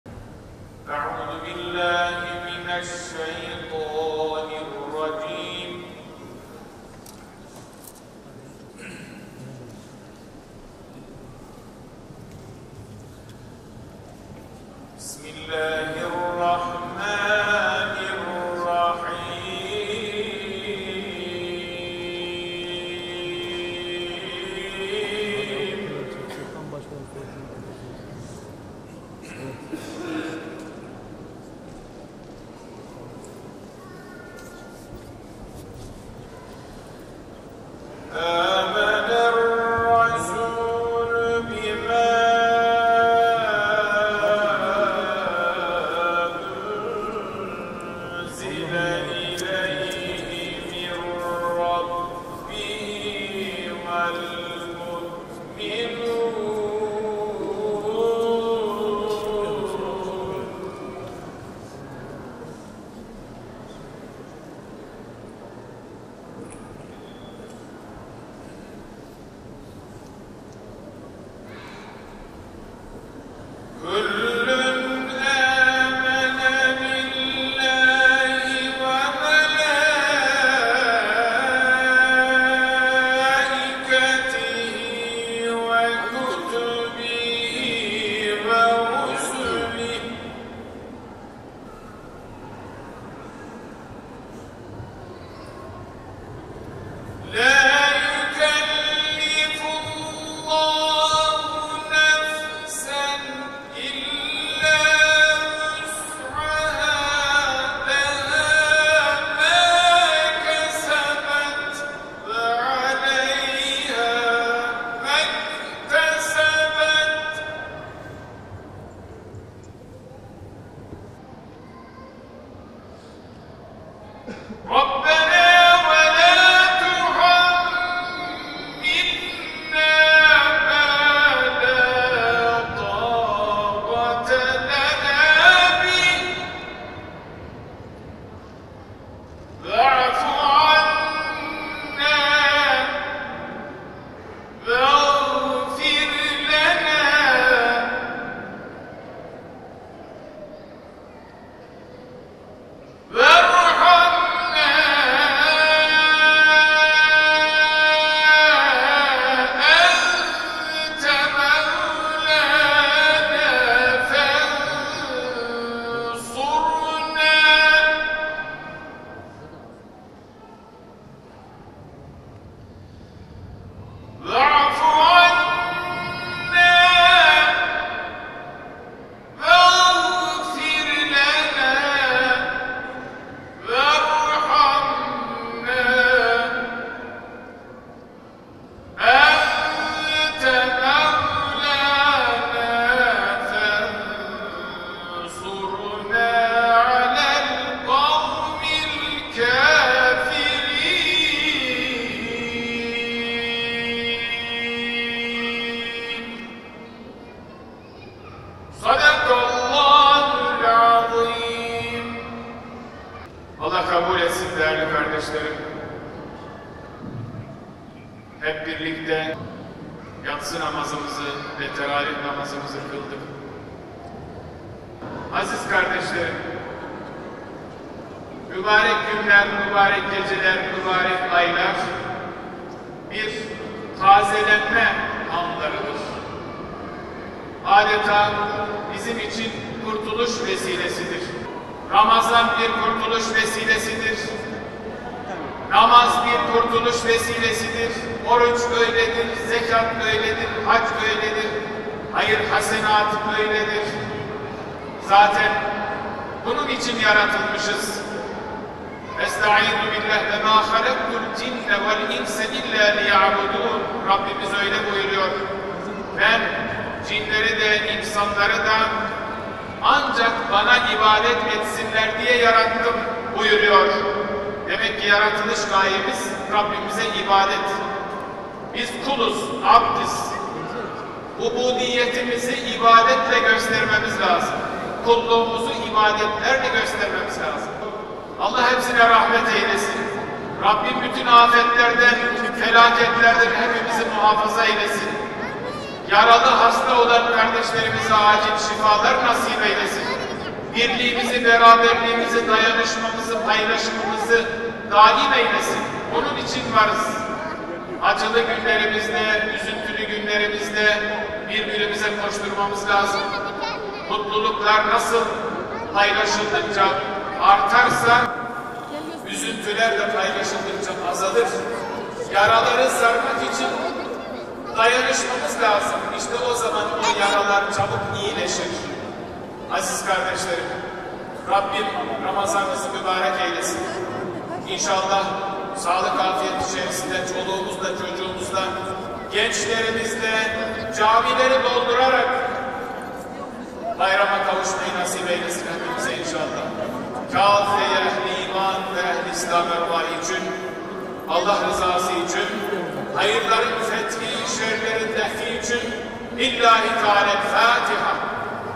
Diyanet İşleri Başkanı Erbaş, Bursa Ulu Camii’nde teravih namazı kıldırdı
Kıldırdığı teravih namazının ardından cemaate hitap eden Başkan Erbaş, mübarek ayların, günlerin ve gecelerin bir kurtuluş vesilesi olduğunu ifade ederek, şöyle konuştu:

“Ramazan, namaz, oruç, zekat ve hac bir kurtuluş vesilesidir, hayır hasenat böyledir. Zaten bunun için yaratılmışız. Rabbim öyle buyuruyor; ‘Ben cinleri de insanları da ancak bana ibadet etsinler diye yarattım.’ Demek ki yaratılış gayemiz Rabbimize ibadet. Biz kuluz ve ubudiyetimizi ibadetle göstermemiz lazım. Kulluğumuzu ibadetlerle beslememiz lazım.”

Başkan Erbaş, depremlerde vefat edenlere Allah’tan rahmet, yaralananlara da acil şifalar dileyerek, “Rabbim birliğimizi, beraberliğimizi, dayanışmamızı, paylaşımımızı daim eylesin. Acılı ve üzüntülü günlerimizde birbirimize koşturmamız lazım. Mutluluklar nasıl paylaşıldıkça artarsa üzüntüler de paylaşıldıkça azalır. Yaraları sarmak için dayanışmamız lazım. İşte o zaman yaralar çabuk iyileşir.” ifadelerini kullandı.